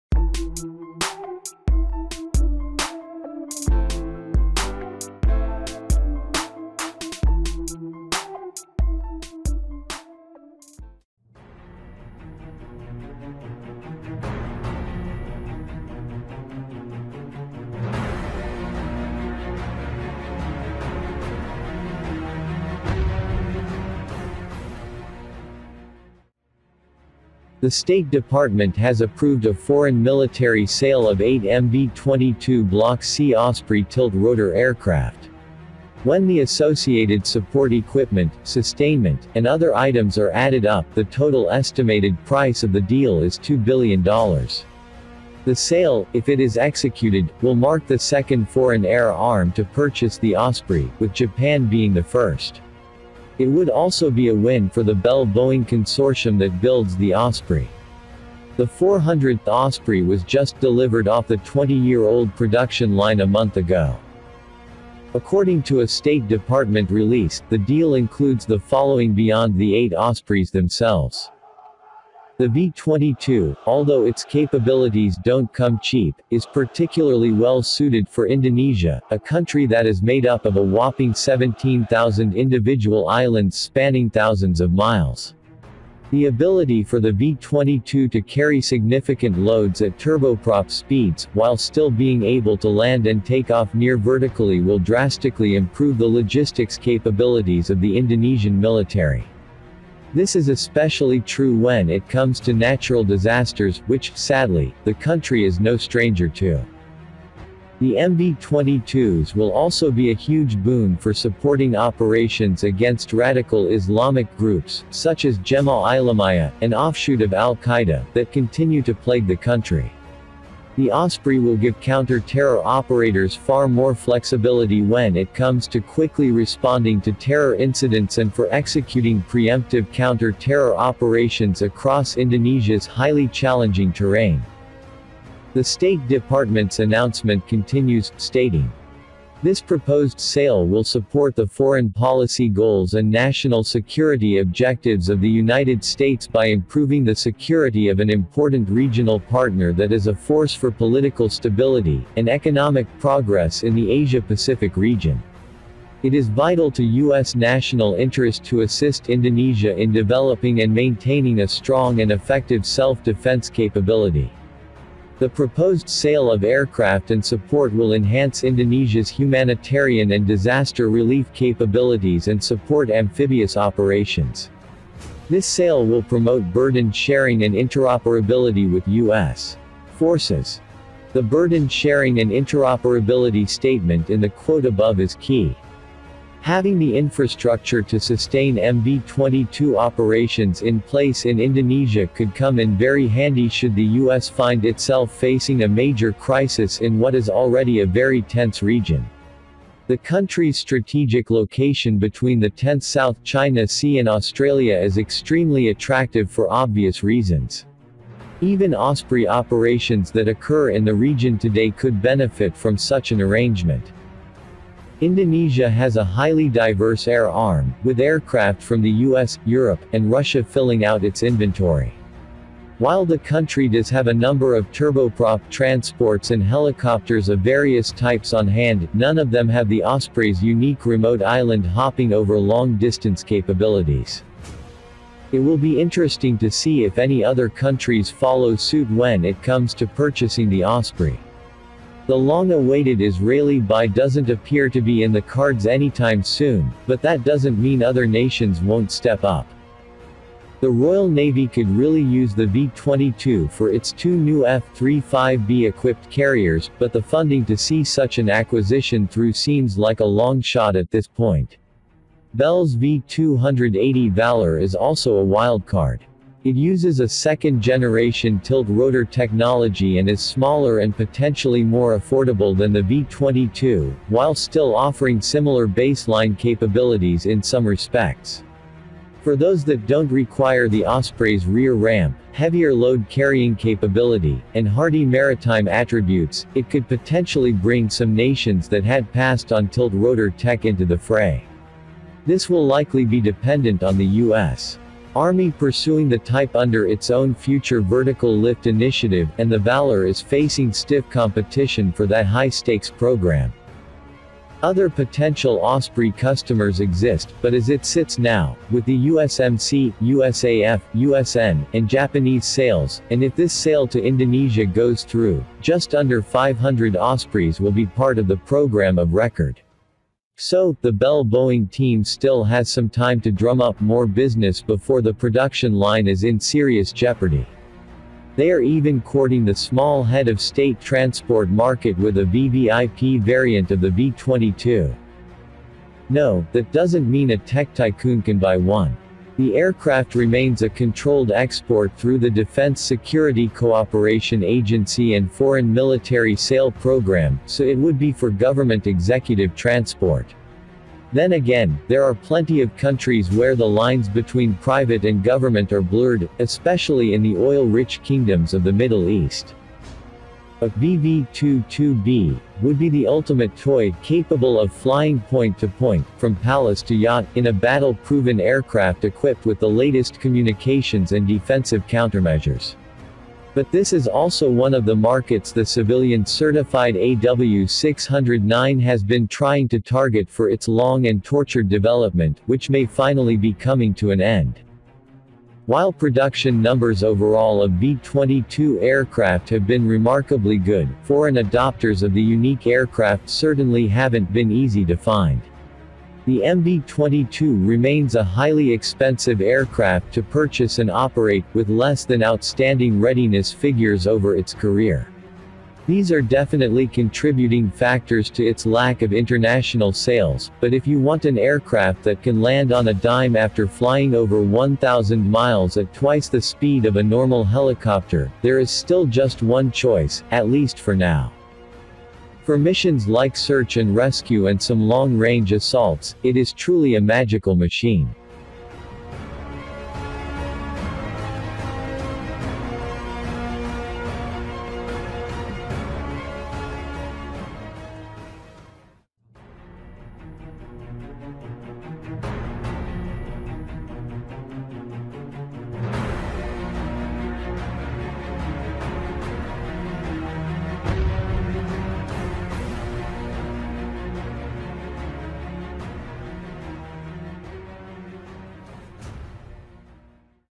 GOOD NEWS: Indonesia cleared to buy 8 MV-22 Ospreys
According to a State Department Release, the deal includes the following beyond the eight Ospreys themselves:

Twenty-four (24) AE 1107C Rolls Royce Engines; twenty (20) AN/AAQ-27 Forward Looking InfraRed Radars; twenty (20) AN/AAR-47 Missile Warning Systems; twenty (20) AN/APR-39 Radar Warning Receivers; twenty (20) AN/ALE-47 Countermeasure Dispenser Systems; twenty (20) AN/APX-117 Identification Friend or Foe Systems (IFF); twenty (20) AN/APN-194 Radar Altimeters; twenty (20) AN/ARN-147 VHF Omni­Directional Range (VOR) Instrument Landing System (ILS) Beacon Navigation Systems; forty (40) ARC-210 629F-23 Multi-Band Radios (Non-COMSEC); twenty (20) AN/ASN-163 Miniature Airborne Global Positioning System (GPS) Receivers (MAGR); twenty (20) AN/ARN-153 Tactical Airborne Navigation Systems; twenty (20) Traffic Collision Avoidance Systems (TCAS II); twenty (20) M-240-D 7.64mm Machine Guns; twenty (20) GAU-21 Machine Guns; Joint Mission Planning Systems (JMPS) with unique planning components; publications and technical documentation; aircraft spares and repair parts; repair and return; aircraft ferry services; tanker support; support and test equipment; personnel training and training equipment; software; U.S. Government and contractor engineering, logistics, and technical support services; and other elements of technical and program support.